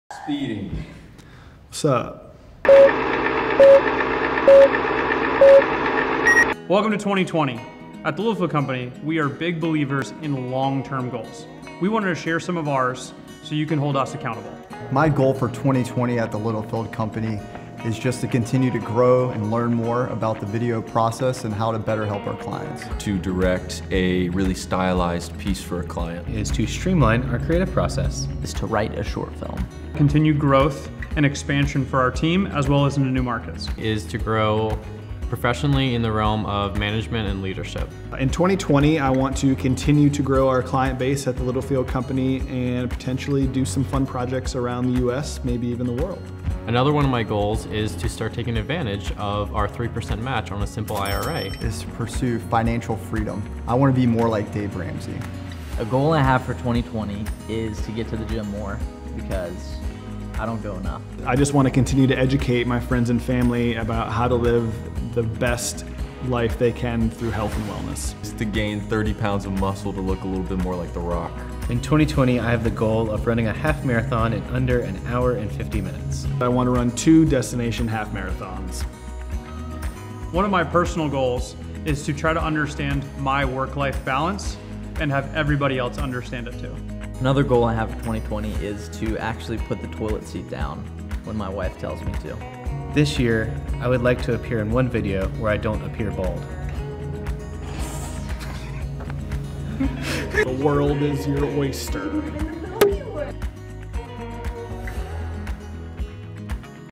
Our 2020 Goals
This year we challenged the team to come up with their goals for the new year. Some of their goals were inspiring, some were audacious and most were just plain weird.